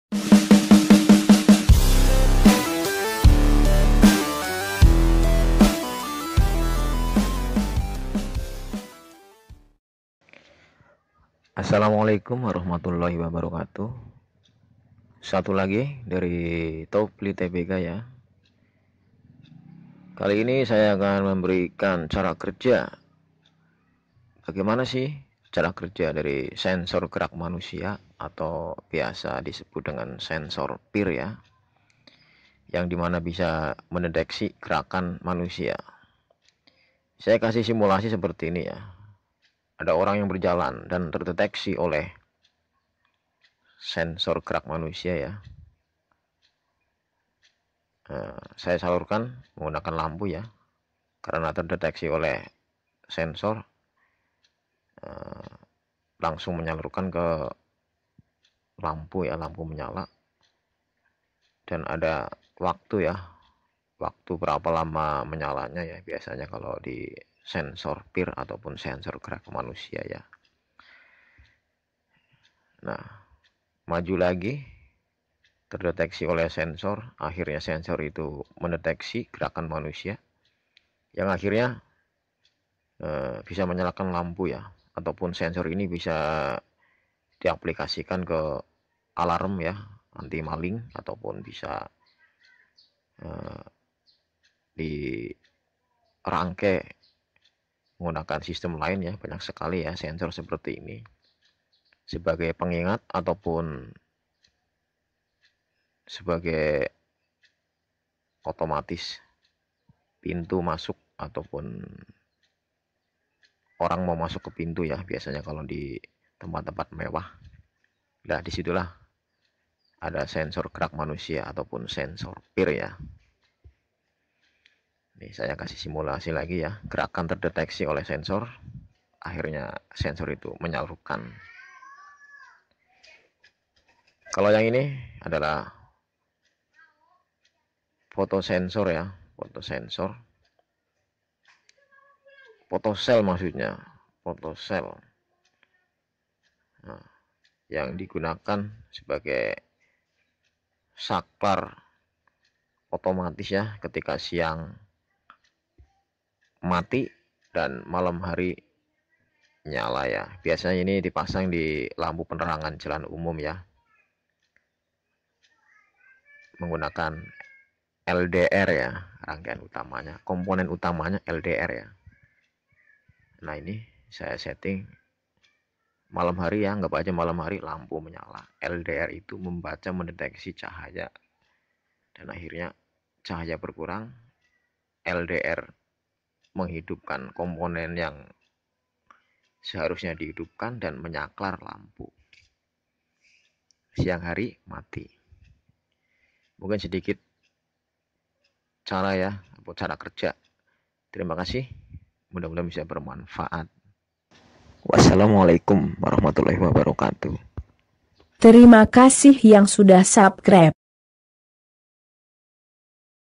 Bagaimana cara kerja sensor gerak//sensor cahaya
ini adalah simulasi Bagaimana cara kerja sensor gerak_manusia yang menggunakan sensor pir yang bisa mendeteksi gerakan dari manusia ya yang di deteksi suhu badannya dan cara kerja sensor cahaya yang biasa dipasang buat otomatis on off di siang dan malam hari.